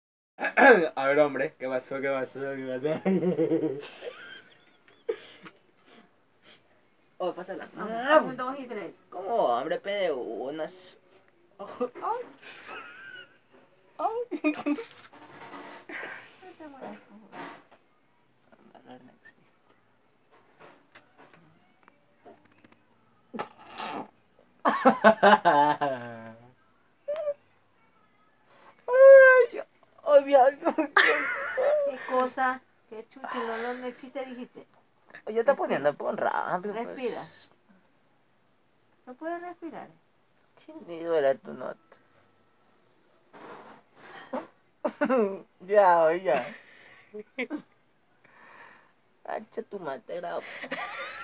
Cuando dice no duele
Puesta de inyección,  haciéndose el que aguanta